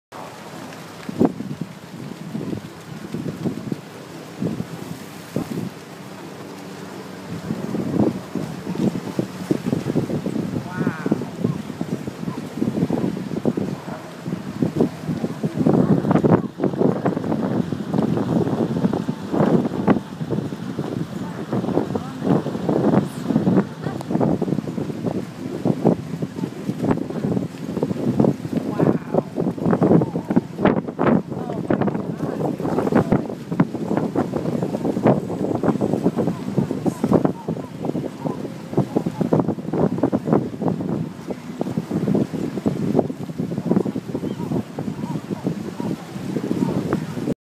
Aerial Attack in Marina Del Rey
1000s of Pelicans, different cormorants and terns pummel the water inside MDR channel. Lasted several hours.